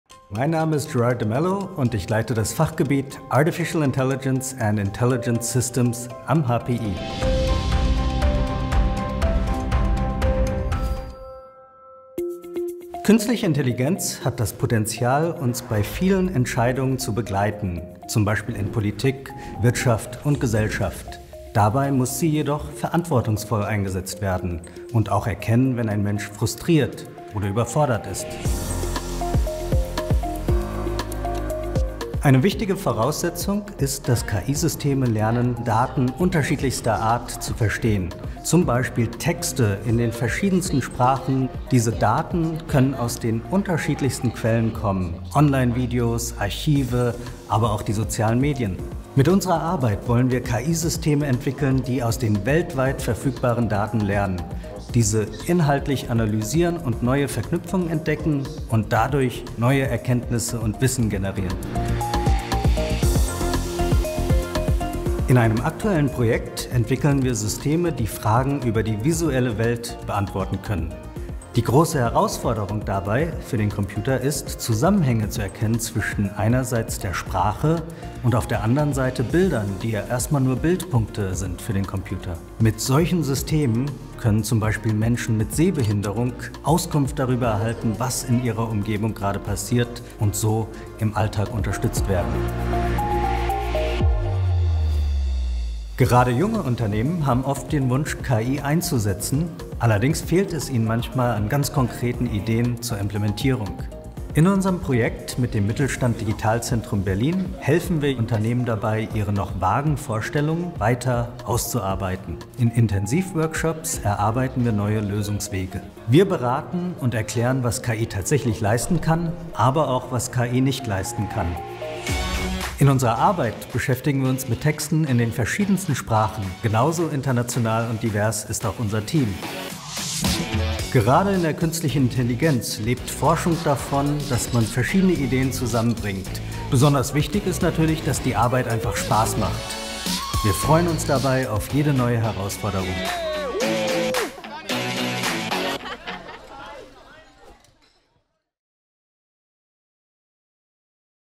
Artificial Intelligence and Intelligent Systems: HPI-Fachgebiet | HPI-TV
Das HPIFachgebiet "Artificial Intelligence and Intelligent Systems", das von Prof. Dr. Gerard de Melo geleitet wird, entwickelt maschinelle Verfahren, mit denen relevante Daten besser verstanden und strukturiert werden können. Dies erfolgt unter Herannahme textueller und weiterer heterogener Datenquellen, auf deren Grundlage intelligente Systeme neue Verbindungen erkennen, Sprach- und Wissenskompetenzen erlernen, sowie letztendlich dem Menschen fundiertere Entscheidungen ermöglichen sollen.